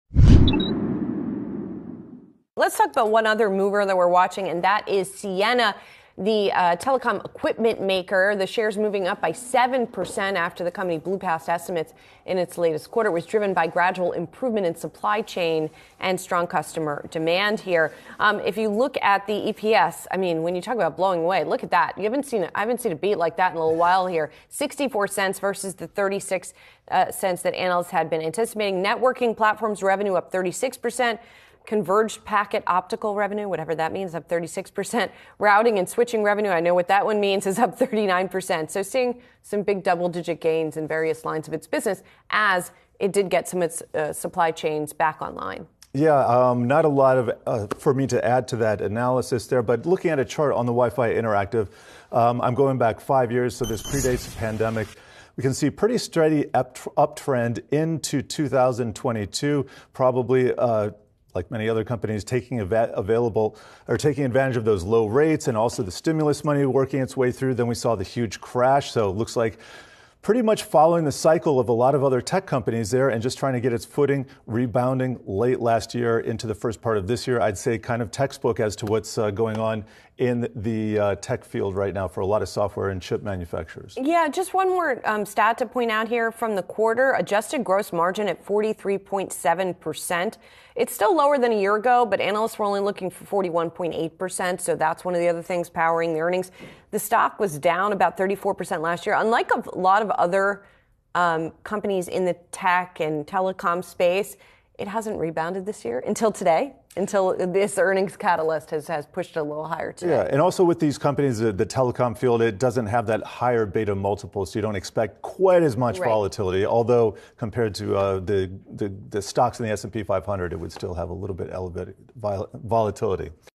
Ciena stock surges on Q1 earnings beat
Yahoo Finance Live anchors Julie Hyman and Jared Blikre discuss Ciena quarterly earnings.